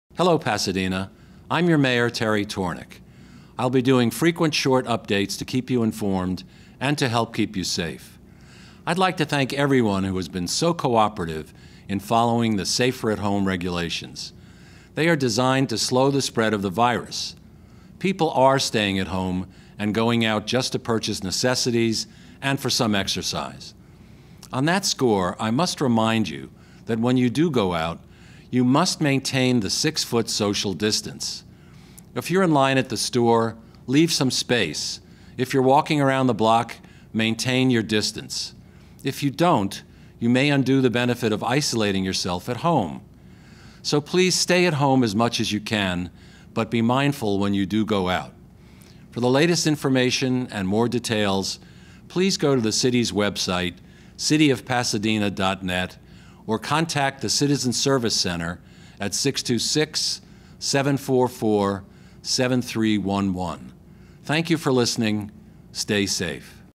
Message from the Mayor - March 24, 2020
Mayor Tornek urges you to stay home as much as possible to help stop the spread of COVID-19. When out for essentials or exercise, be mindful and maintain a distance of 6 feet or more between you and others in the community.